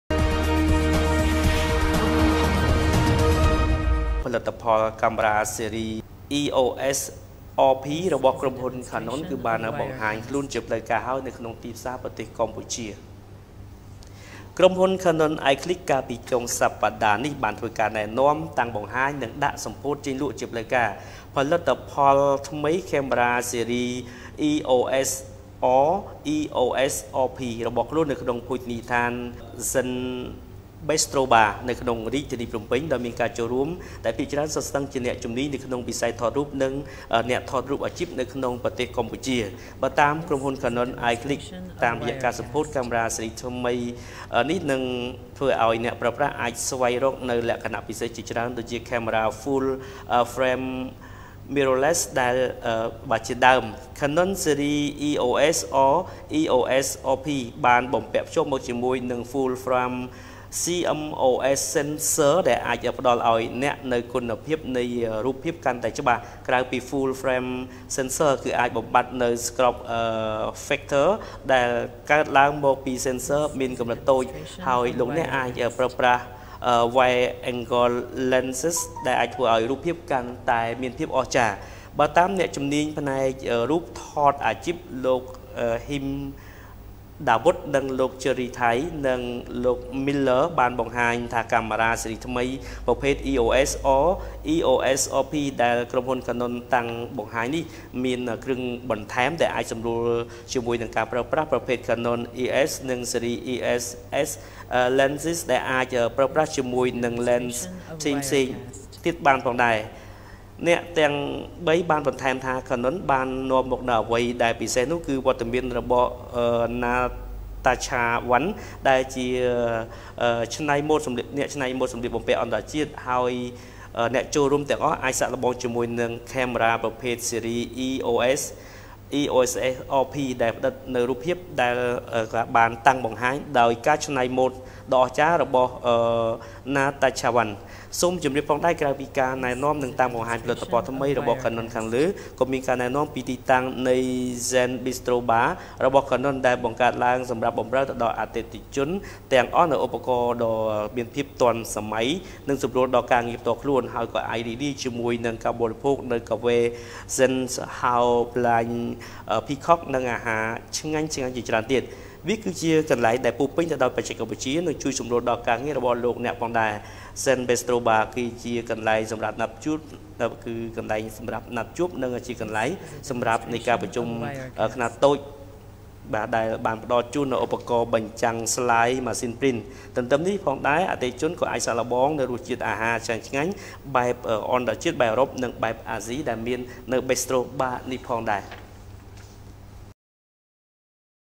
កាំមេរ៉ា ស៊េរី EOS R/ EOS RP របស់ក្រុមហ៊ុនកាណុន បានបង្ហាញខ្លួន ជាផ្លូវការ
ផលិតផល កាំមេរ៉ា ស៊េរី EOS R/ EOS RP របស់ក្រុមហ៊ុនកាណុន បានបង្ហាញខ្លួន ជាផ្លូវការហើយនៅលើទីផ្សារប្រទេសកម្ពុជា
ក្រុមហ៊ុន Canon I-Qlick កាលពីចុងសប្តាហ៍នេះ បានធ្វើការណែនាំ តាំងបង្ហាញ និងដាក់ សម្ពោធចេញលក់ជាផ្លូវការ ផលិតផលថ្មី កាំមេរ៉ា ស៊េរី EOS R/EOS RP របស់ខ្លួន នៅក្នុងភោជនីយដ្ឋាន SENZE Bistro Bar ក្នុងរាជធានីភ្នំពេញ ដោយមានការចូលរួម ដែលភាគច្រើនសុទ្ធសឹងជាអ្នកជំនាញក្នុង វិស័យថតរូប និងជាអ្នកថតរូបអាជីពនៅក្នុងប្រទេសកម្ពុជា។
បើតាមក្រុមហុន កាណុន អាយឃ្លីក,តាមរយៈការសម្ពោធ កាំមេរ៉ាស៊េរីថ្មី EOS R/EOS RP នេះ អ្នកប្រើប្រាស់អាច ស្វែងរក នូវលក្ខណៈ ពិសេសជាច្រើនដូចជា ប្រភេទកាំមេរ៉ា full frame mirrorless ជាដើម។ Canon ស៊េរី EOS R/EOS RP គឺបានបំពាក់ភ្ជាប់មកជាមួយនឹង full frame CMOS sensors ដែលអាចផ្ដល់អោយលោកអ្នកនូវ គុណភាពនៃរូបភាពកាន់តែច្បាស់ ក្រៅពីនេះ full frame sensors អាចបំបាត់នូវ crop factor ដែលកើតឡើងមកពី sensors មានកម្រិតតូច ហើយលោកអ្នកអាចប្រើប្រាស់ wide angel lenses ដែលធ្វើអោយរូបភាពកាន់តែអស្ចារ្យ។
បើតាមអ្នកជំនាញ ផ្នែកថតរូបអាជីព  លោក ហ៊ីម ដាវុធ និងលោក Jerry Thai និង លោក Miller បង្ហាញថា  កាមេរ៉ា ស៊េរីថ្មីប្រភេទ EOS R/EOS RP ដែលក្រុមហ៊ុនកាណុន តាំងបង្ហាញនេះ  មានគ្រឿង បន្ថែម ដែលអាច សម្រួលជាមួយការប្រើ ប្រាស់ ប្រភេទ Canon EF និង ស៊េរី EF-S lenses ដែលអាច ប្រើប្រាស់ជាមួយនឹង lenses ផ្សេងៗផងដែរ។ អ្នកទាំងពីរ បន្ថែមថា Canon បាននាំមកនូវអ្វី ដែលពិសេសនោះគឺ វត្តមានរបស់ Natacha Van ដែលជា ច្នៃម៉ូតសម្លៀកបំពាក់ អន្ដរជាតិ ហើយអ្នកចូលរួមទាំងអស់អាចសាកល្បងជាមួយនឹង កាំមេរ៉ា ប្រភេទថ្មី ស៊េរី EOS R/EOS RP  ដោយផ្តិតនូវ រូបភាព ដែលបានតាំងបង្ហាញ ដោយការឆ្នៃម៉ូតដ៏អស្ចារ្យរបស់ Natacha Van។
សូមជំរាបផងដែរ ក្រៅពីការណែនាំនិងតាំងបង្ហាញ ផលិតផលថ្មី របស់កាណុន ខាងលើនេះ ក៏មាន ការណែនាំ ពីទីតាំង នៃ SENZE Bistro Bar របស់កាណុន ដែលបង្កើតឡើងសម្រាប់បម្រើ អតិថិជន ទាំងអស់ នូវឧបករណ៍ទាន់សម័យ និងសម្រួលដល់ ការងារផ្ទាល់ខ្លួន ហើយក៏អាចរីករាយជាមួយ នឹងការ បរិភោគនូវ កាហ្វេ SENZE house blend Peacock និងអាហារ ឆ្ងាញ់ៗជាច្រើនទៀត។ វាគឺជាកន្លែង ដែលពោពេញទៅដោយបច្ចេកវិទ្យា និងជួយសម្រួលដល់ការងាររបស់លោកអ្នកផងដែរ ។ SENZE Bistro Bar ក៏ជាកន្លែង សម្រាប់ណាត់ជួប និង ជាកន្លែង សម្រាប់ការប្រជុំខ្នាតតូច ដែលមានផ្ដល់ជូននូវ ឧបករណ៍បញ្ចាំងslide ម៉ាស៊ីនព្រីន។ រទន្ទឹមនិងនេះផងដែរ អតិថិជនក៏អាចសាកល្បងនូវរស់ជាតិ អាហារឆ្ងាញ់ៗ បែបអន្ដរជាតិ បែបអ៊ឺរ៉ុប និងបែបអាស៊ី ផងដែរ។
បើតាម អ្នកគ្រប់គ្រង SENZE Bistro Bar មាន មុខម្ហូបបែប អាស៊ី ដែលមានផ្ដល់ជូនដូចជា Fried Hokkien Noodlles Fried Carrot cake និង Laksa ហើយនិងម្ហូបបែបអាស៊ី ឆ្ងាញ់ៗជាច្រើន និងម្ហូបបែប អ៊ឺរ៉ុប ដែលមានដូចជា Char Grilled Chicken cutlet ជាមួយនឹង Green Pepper និងមាន Spaghetti Carbonara ផងដែរ។ជាមួយនឹងកាហ្វេ ក្នុងស្រុក អតិថិជន ក៏អាចរីករាយជាមួយនឹងកាហ្វេ ពិសេស Peacock brand ដែលមានលក់នៅក្នុង SENZE Bistro Bar និង Ice blended Pineapple Tart Freeze និង Oreo Black forest ដែលយើងទាំងអស់គ្នាគួរតែសាកល្បង៕